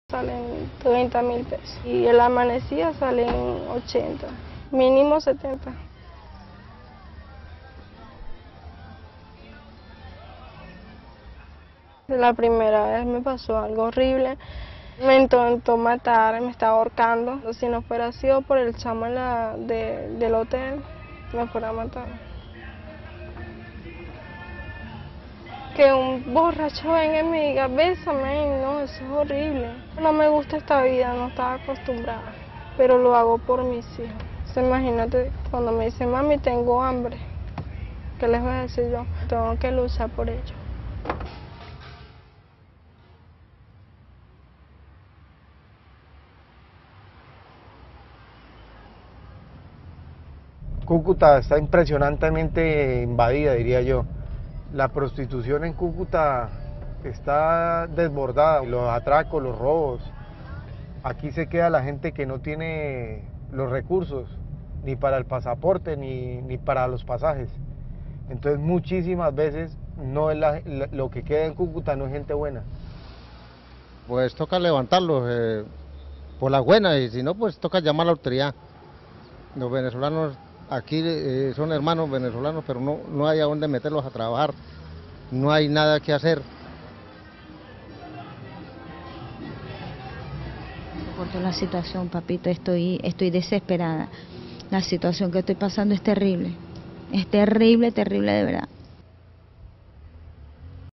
NOS | Dayana tippelt uit pure wanhoop in Colombia
Het is zaterdagnacht, topdrukte in de rosse buurt van de Colombiaanse grensstad Cúcuta. Gele taxi's rijden af en aan. Langs de kant van de weg staan groepjes vrouwen in weinig verhullende kleding. De meesten komen uit Venezuela.

Een van hen is de 21-jarige Dayana. "Het was niet vol te houden in Venezuela. Mijn moeder heeft medicijnen nodig, maar die zijn niet meer te betalen. En mijn kinderen moeten eten." Daarom besloot ze te emigreren, in de hoop in Colombia een baan te vinden waarmee ze haar familie kan onderhouden.